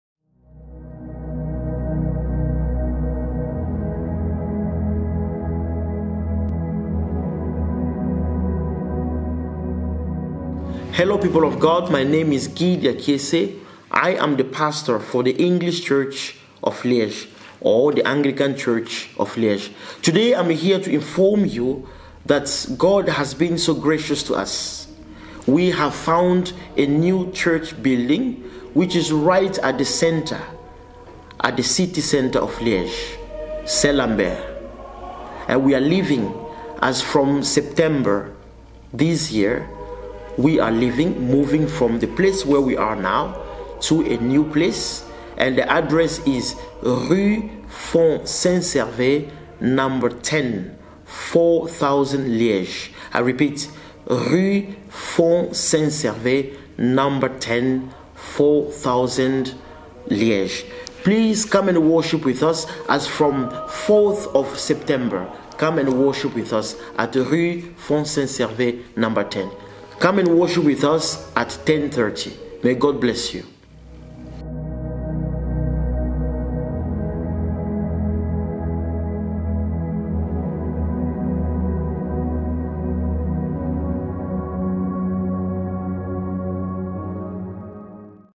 Anglican church of Liège - our new church building
Welcome to our new church...
Our first service will be: Sunday, September 4, 2022 at 10:30 a.m.
Rue Fond Saint-Servais 10
4000 Liège
Belgique